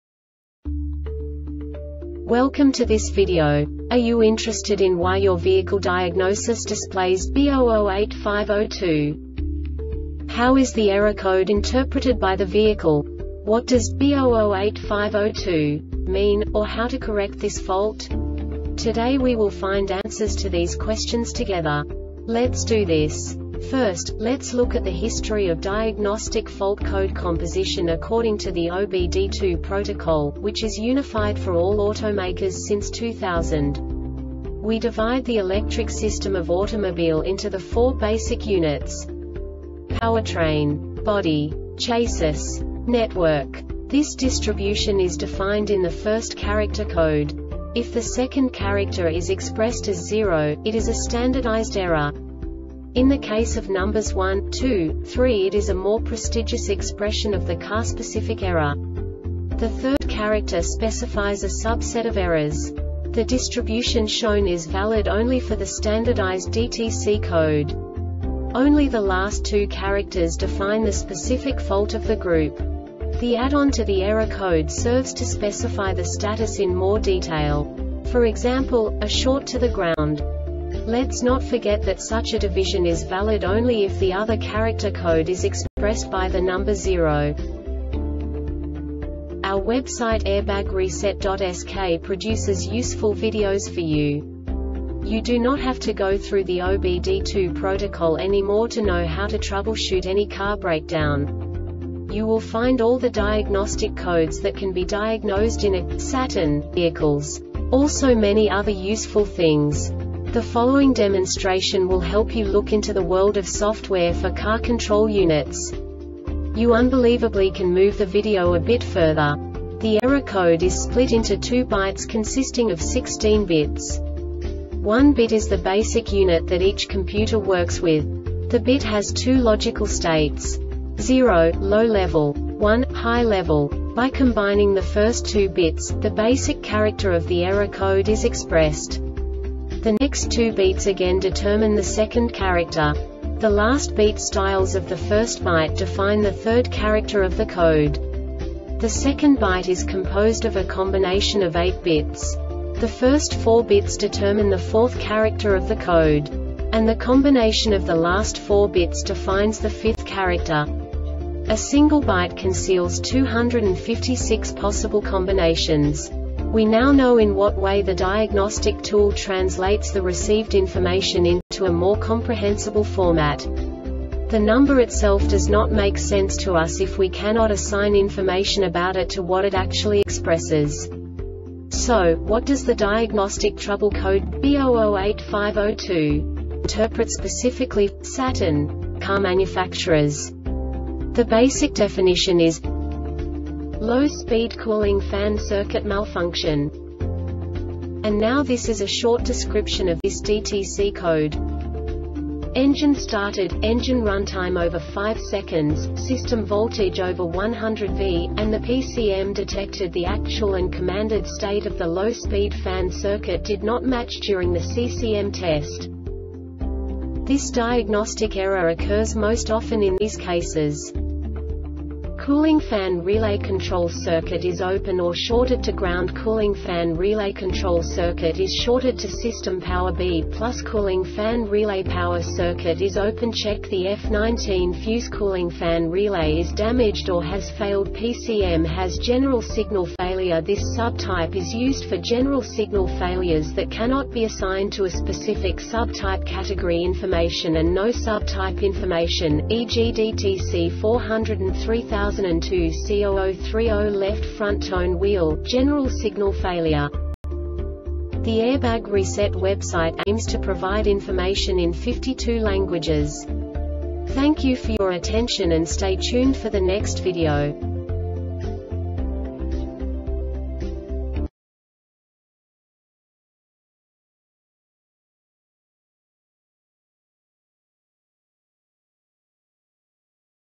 DTC Saturn B0085-02 Short Explanation
Contents:
0:21 Basic DTC analysis according to OBD2 protocol standard.
1:48 Insight into programming
2:58 A diagnostically understandable description of the Diagnostic Trouble Code
3:05 Basic error definition

Make:
Saturn
Code:
B0085 02
Definition:
Low Speed Cooling Fan Circuit Malfunction
Description:
Engine started,engine runtime over 5 seconds,system voltage over 10.0v,and the PCM detected the Actual and Commanded state of the Low speed fan circuit did not match during the CCM test.
Cause:
Cooling fan relay control circuit is open or shorted to groundCooling fan relay control circuit is shorted to system power (B+)Cooling fan relay power circuit is open (check the F19 fuse)Cooling fan relay is damaged or has failedPCM has
Failure Type: